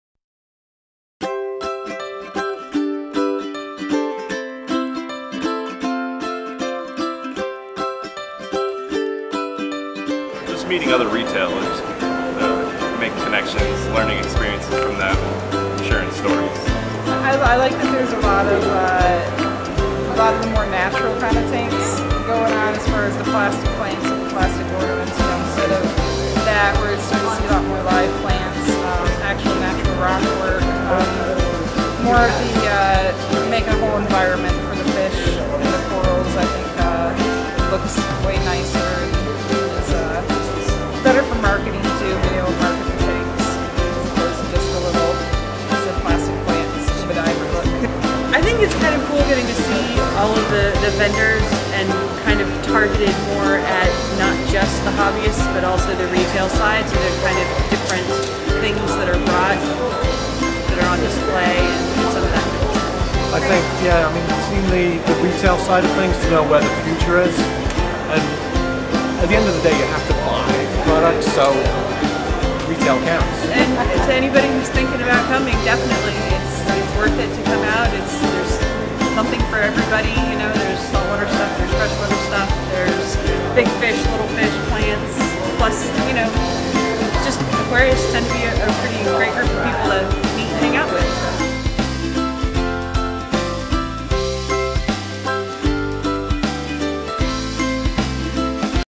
The Aquatic Experience 2017 - What Do You Love Most About the Aquatic Experience
Aquatic Experience – Chicago is the first show in our industry’s history to combine everything aquatic under one roof. From saltwater to freshwater with pond and aquatic reptiles, we have it all. See what our event-goers have to say.